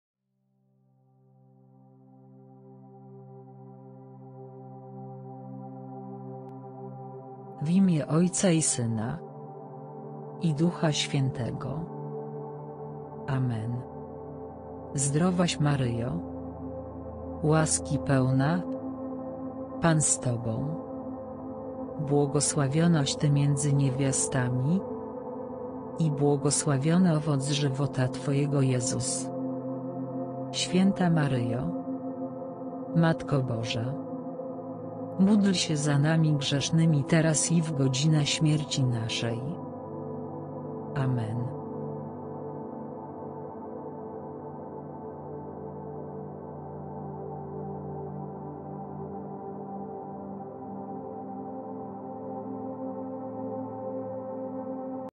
🙏 Modlitwa Zdrowaś Maryjo - Pozdrowienie Anielskie 🙏
Modlitwa Zdrowaś Maryjo - Pozdrowienie Anielskie: tradycyjna oparta na Piśmie Świętym chrześcijańska modlitwa uwielbienia i prośby do Najświętszej Maryi Panny. Módl się z nami 🙏!

Modlitwa Zdrowaś Maryjo (Tekst):

Zdrowaś Maryjo, łaski pełna, Pan z Tobą,
błogosławionaś Ty między niewiastami,
i błogosławiony owoc żywota Twojego, Jezus.

Święta Maryjo, Matko Boża,
módl się za nami grzesznymi teraz i w godzinę śmierci naszej.

Amen.

Mamy nadzieję, że film Ci się spodoba.

Zachęcamy do pisania swoich intencji modlitewnych w komentarzach.

Dziękujemy za Twoje wsparcie! Niech Cię Bóg błogosławi!